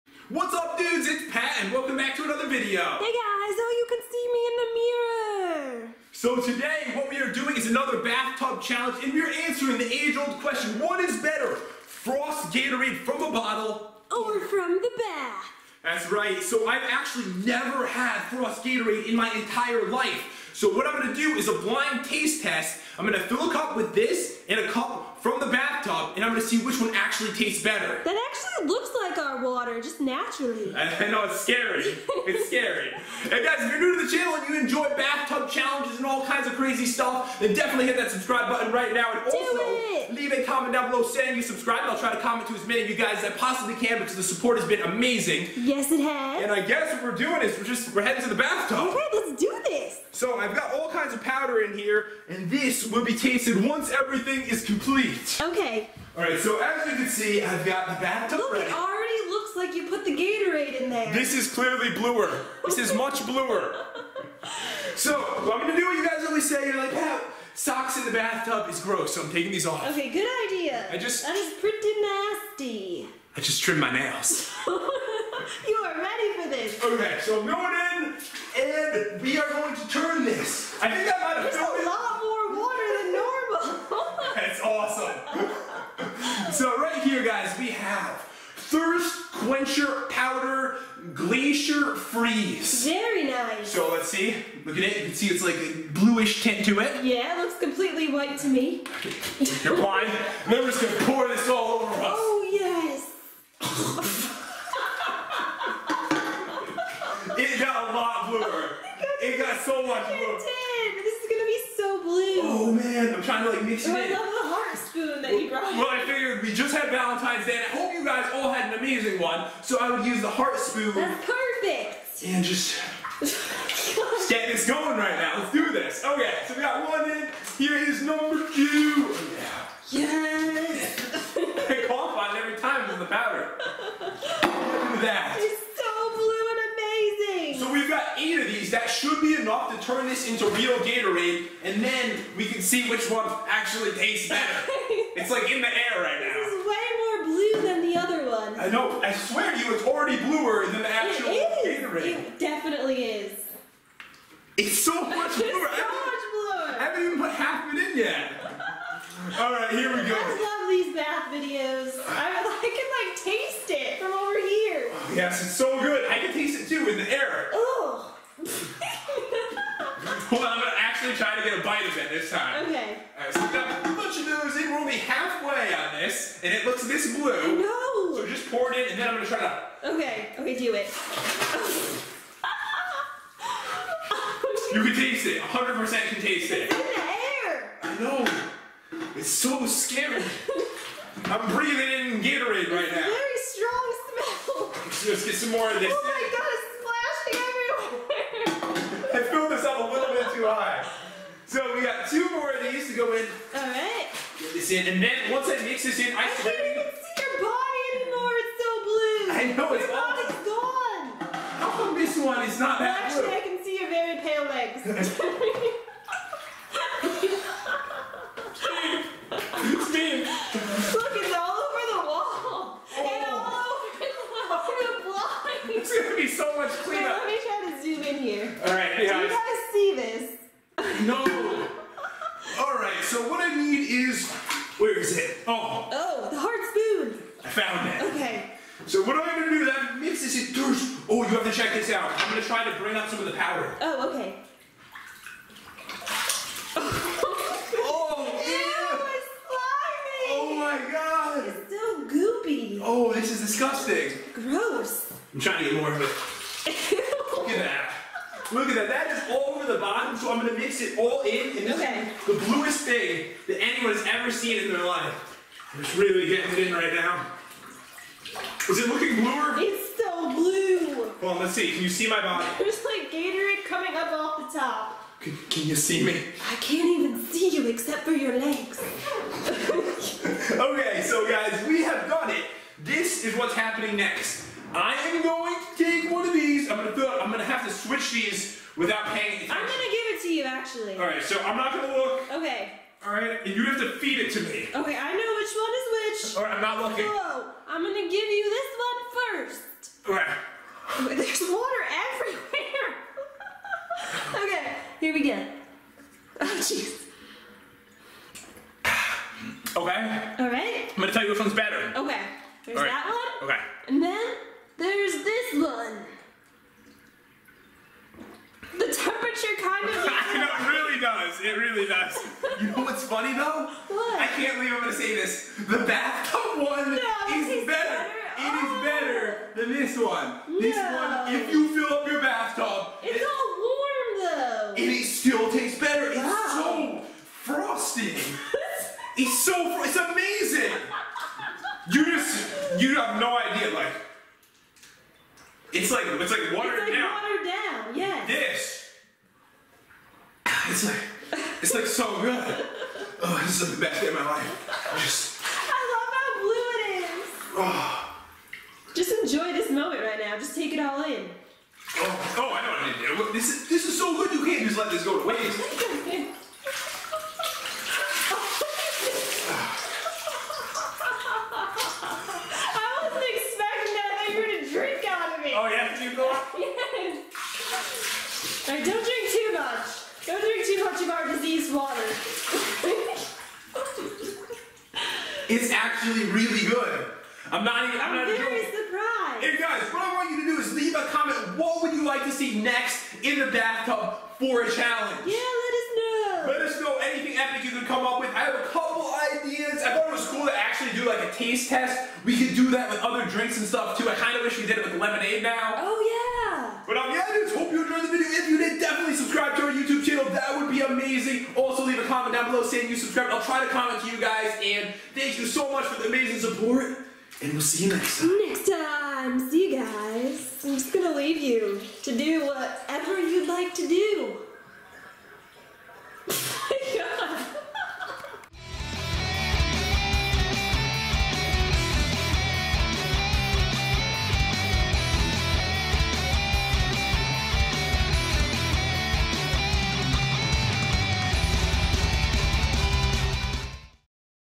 BLUE GATORADE BATH CHALLENGE!!!
Today I am taking a bath in blue frost Gatorade!!

In this video we are doing a bath experiment with gatorade. We are mixing it together and hopefully turning it into a real drink in this challenge! Then we are taste testing it against the real thing!